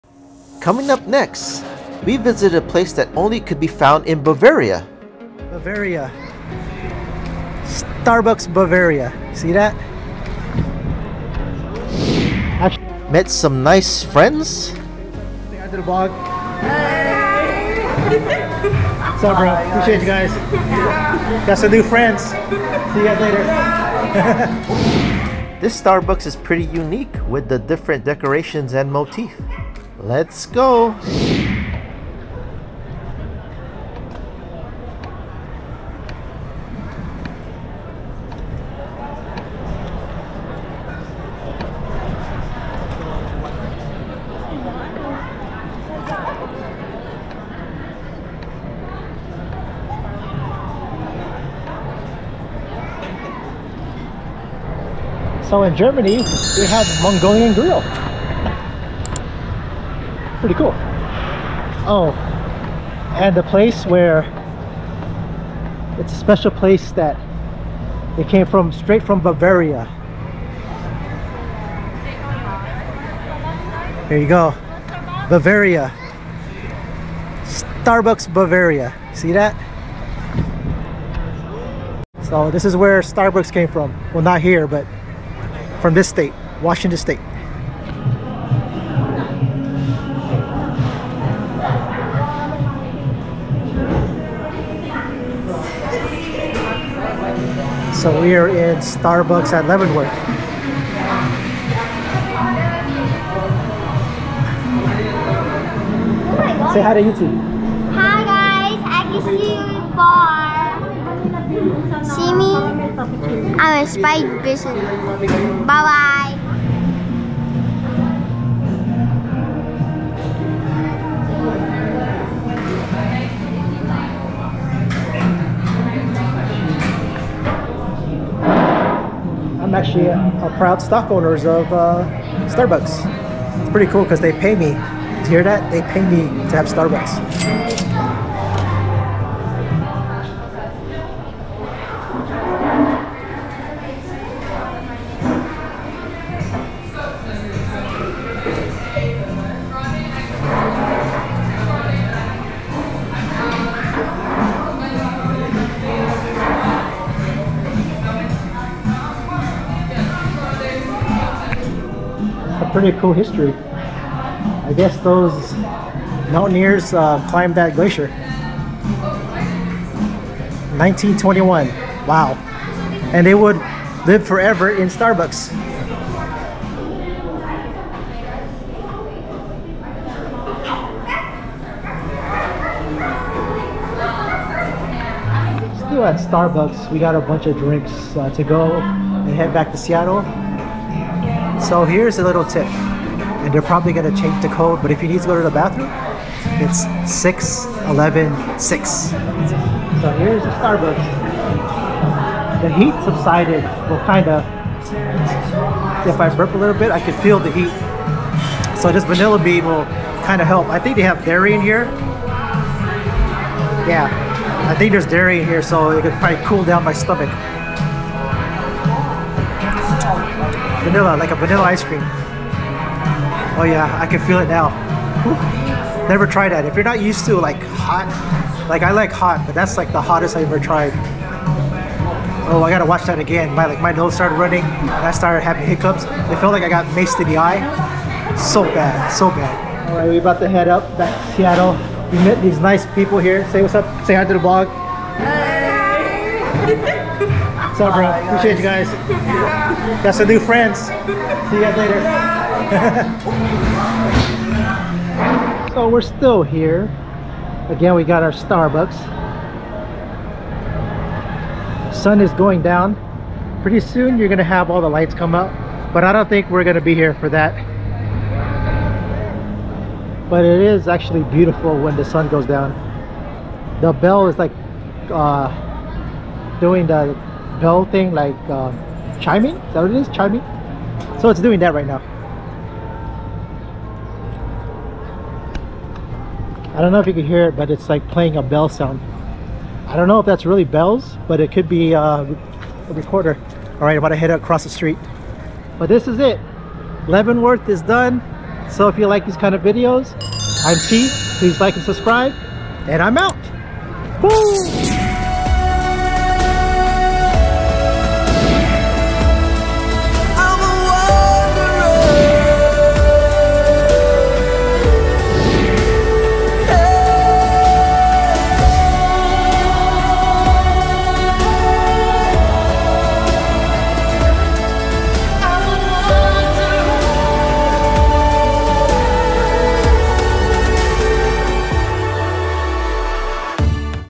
Caffeine Bliss: Leavenworth Starbucks in Washington 4K
Calling all adventure-seekers and coffee enthusiasts alike! Buckle up for an epic journey as we explore the captivating world of Leavenworth Starbucks in Washington State. But wait, you might be wondering, 'I don't drink coffee, is this vlog for me?' Fear not, my fellow non-coffee connoisseurs, because Leavenworth Starbucks has something delightful in store for everyone!

Join me as we step into a caffeine wonderland where aroma reigns supreme and baristas are the true wizards of taste. From their signature coffee creations to an array of tantalizing tea blends, there's a beverage with your name on it. Prepare to be swept away by the enchanting atmosphere, rich history, and the sheer joy of discovering a true gem nestled in the heart of Leavenworth.

Not a coffee lover, like myself? No problem! Leavenworth Starbucks has an array of delectable treats that will tantalize your taste buds. From freshly baked pastries to mouthwatering snacks, there's a delightful surprise waiting for you at every corner. So, whether you're a coffee aficionado or simply seeking a memorable experience, this vlog is your passport to adventure!